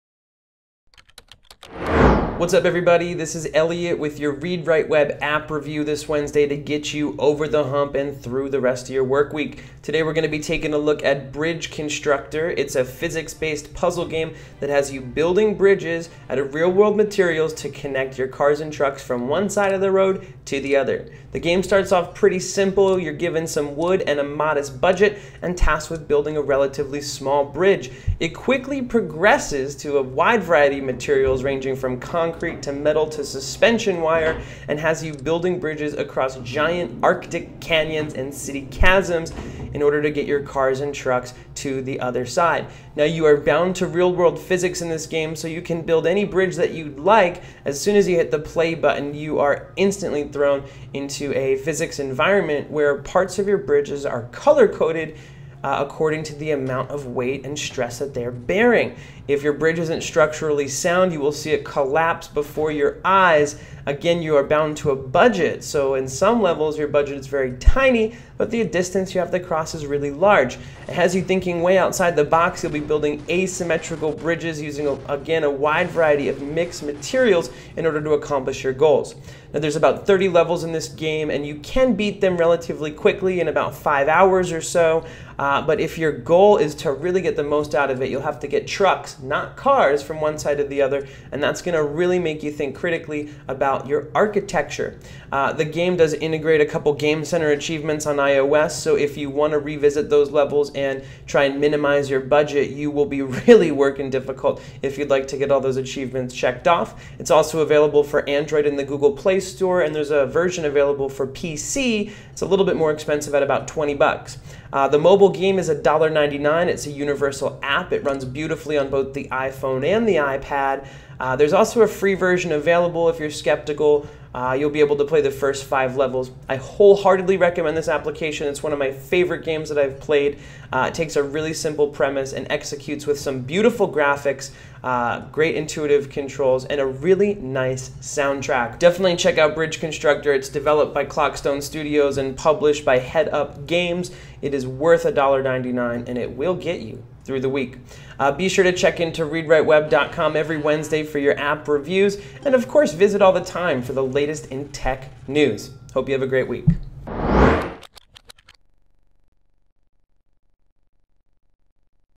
Live Your Structural Engineering Dreams with Bridge Constructor - Hump Day Review
Touch screens have proven fertile ground for physics-based puzzle games.  And while slingshot knocking, match-3 dropping, ragdoll flinging titles tend to dominate the genre- we love one game in particular for abandoning the formula and embracing - you guessed it - constructing bridges.

Bridge Constructor will have you managing budgets and mixing resources in an attempt to get vehicles across gaps of increasing sizes.  Its refreshing simplicity and challenging learning curve make it the perfect hump-day app.

Follow me @eliotweisberg